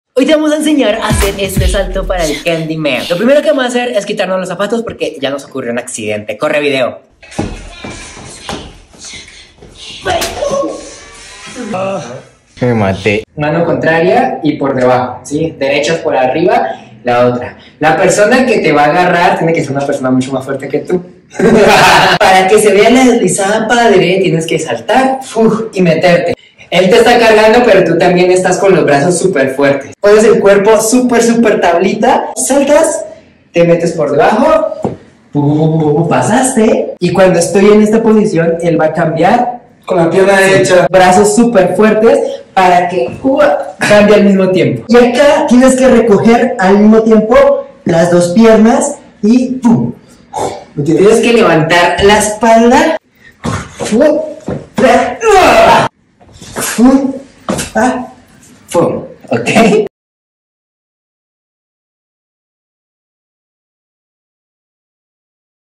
TRUCO de Baile en Pareja 👥 | Tutorial (Facil)
Aprende este truco de baile en pareja paso a paso y Muy Fácil!

A BAILAR!!